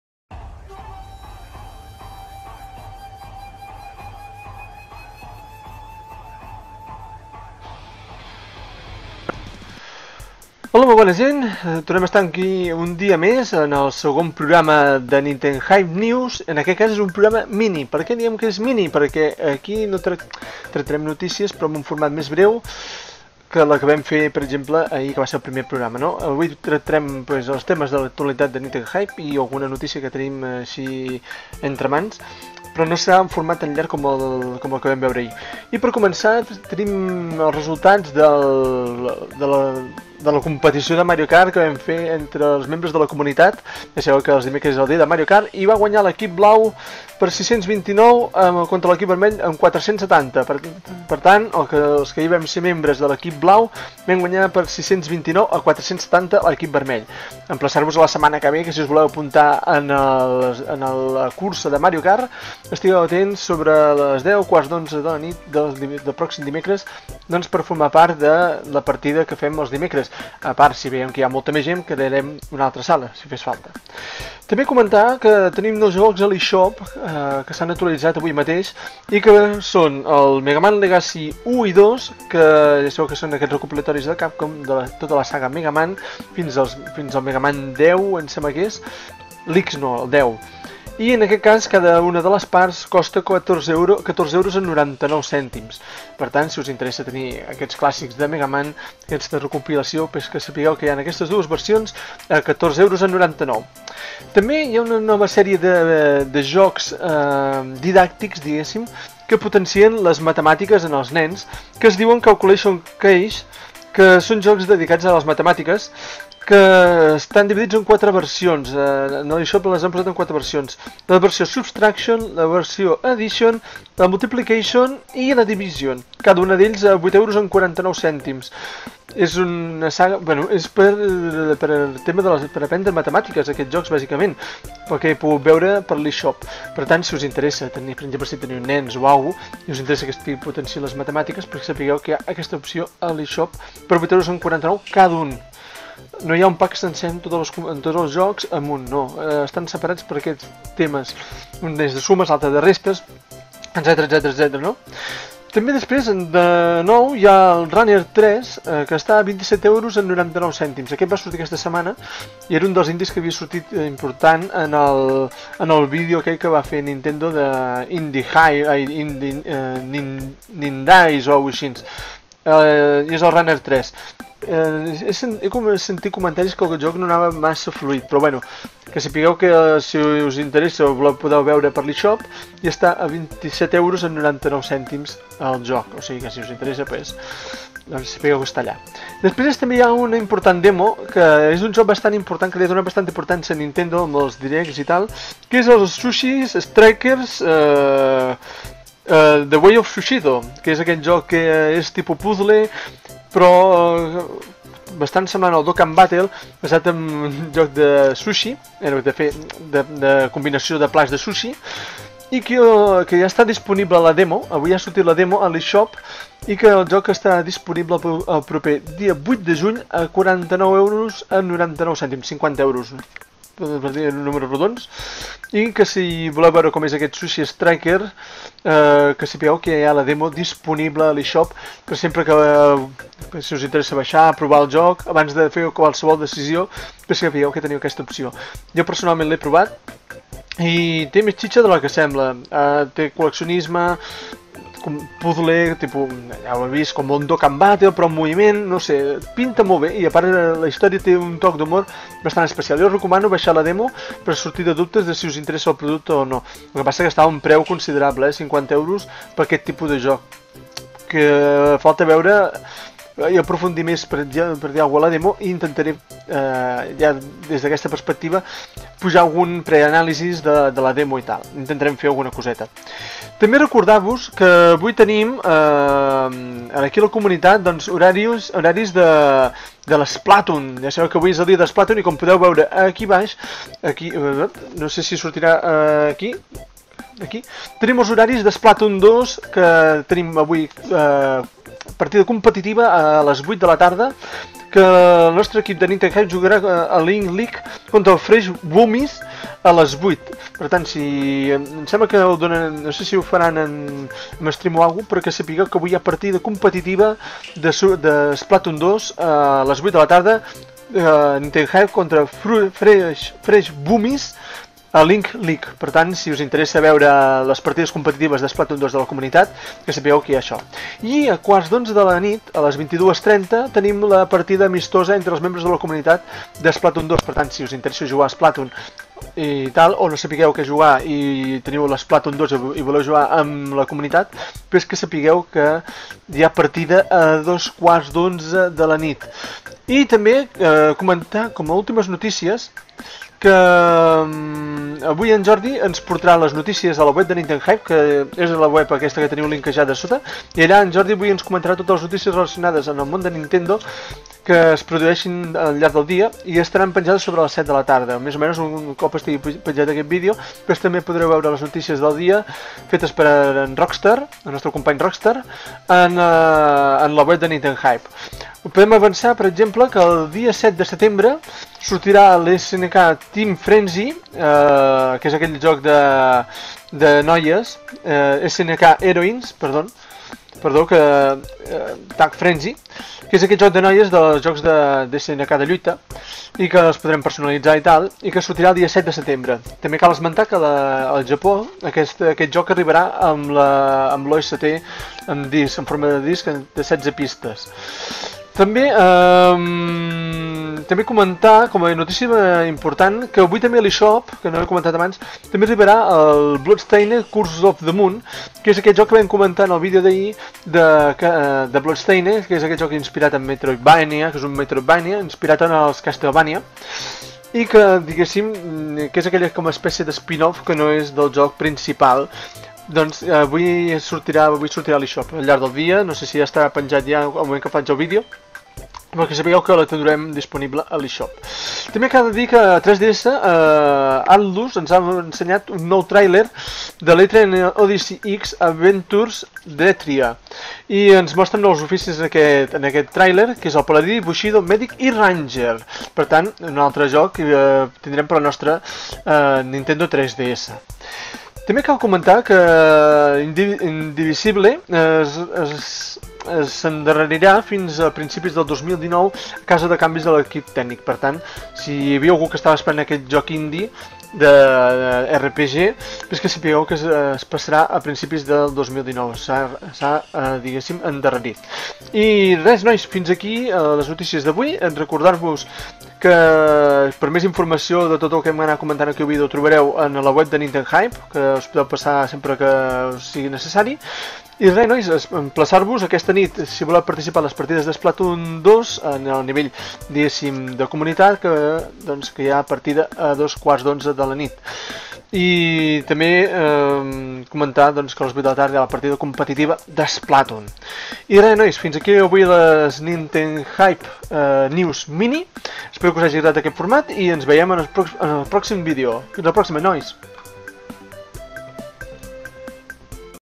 NTHNews Mini: Les notícies del dia (24/05/2018)
Primer programa de Nintenhype News Mini, on repassarem les novetats del dia en clau Nintendo i de la nostra comunitat, repassarem notes de premsa, Resultats dels nostres equips competitius, quedades, anàlisis a la web i molt més...
HYPE!